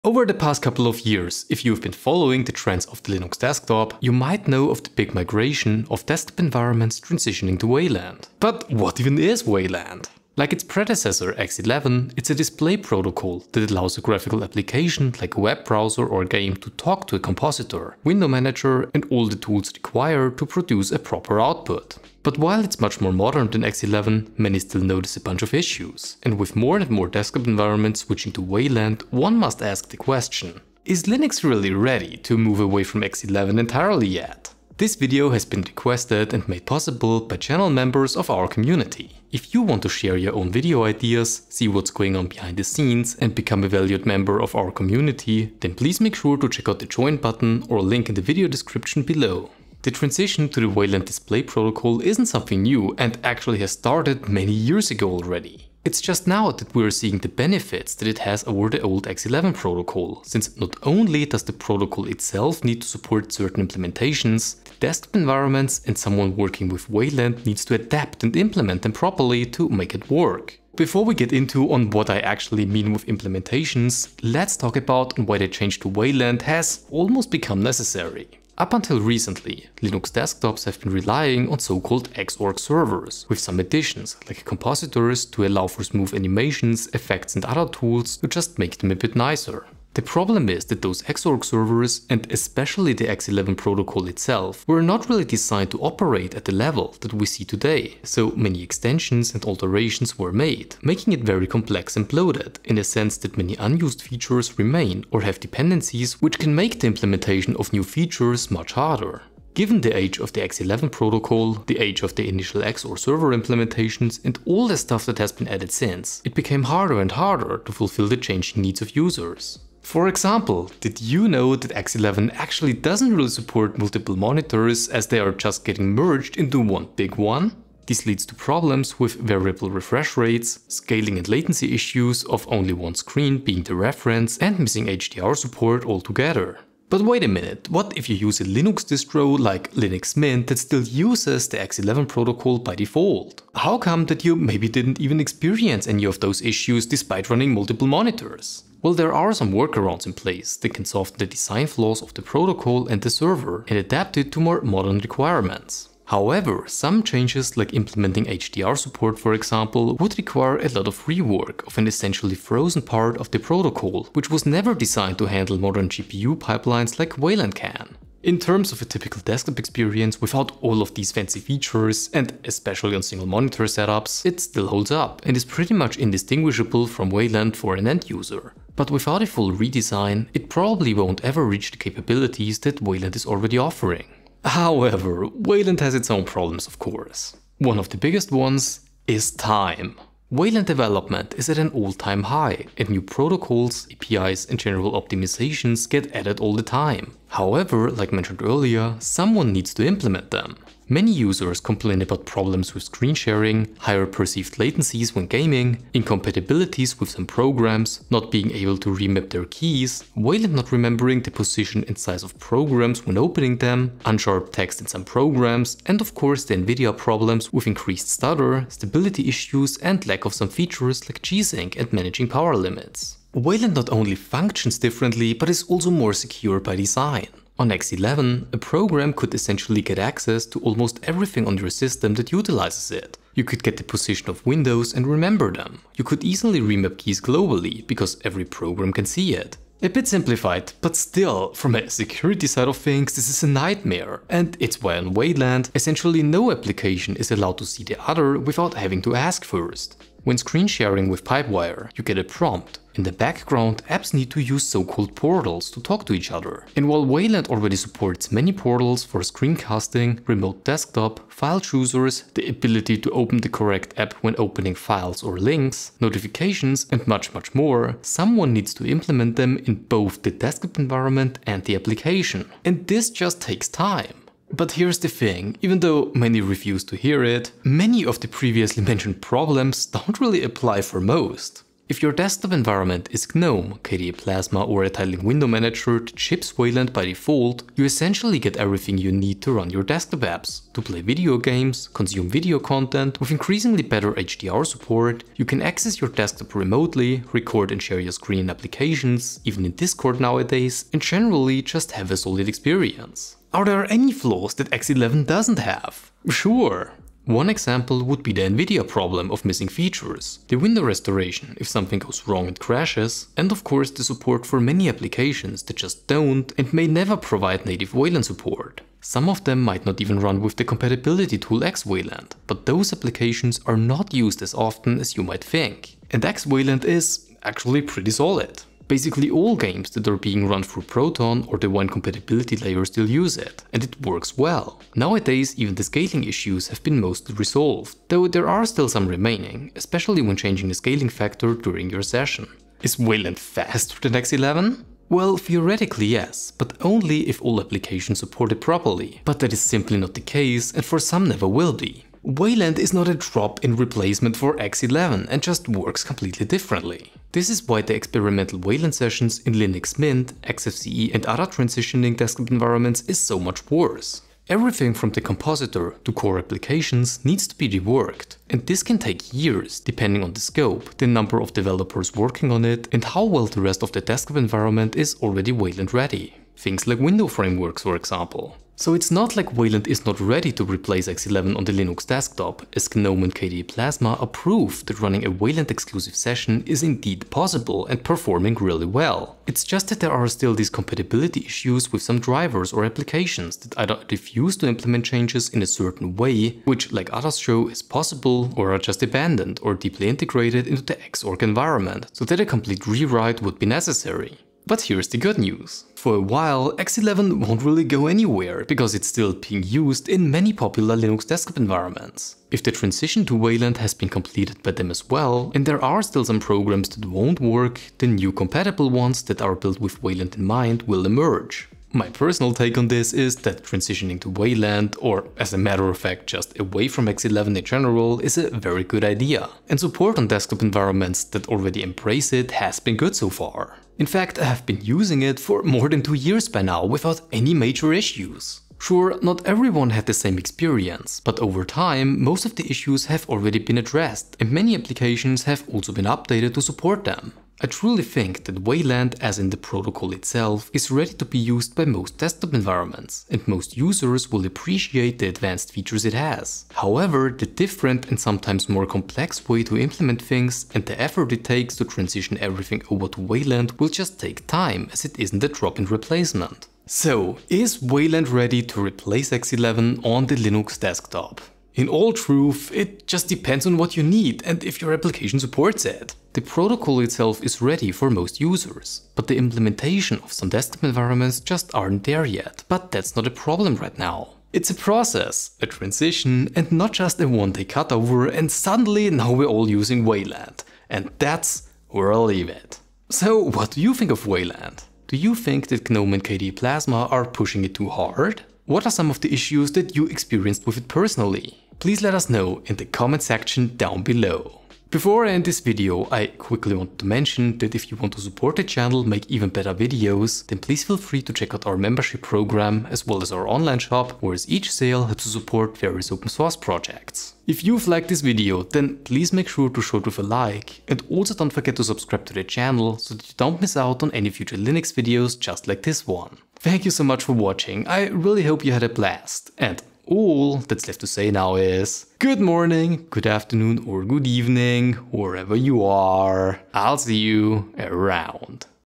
Wayland vs X11: Is It Finally Time to Switch?
► Chapters:

00:00 - The big Wayland Migration
00:49 - It has started a long time ago
01:15 - Why change to Wayland at all?
02:16 - "I have no problems with X11..."
03:09 - Wayland problems
04:33 - It's good enough
05:53 - Wayland is not Wayland ...
07:01 - My personal opinion
07:46 - Conclusion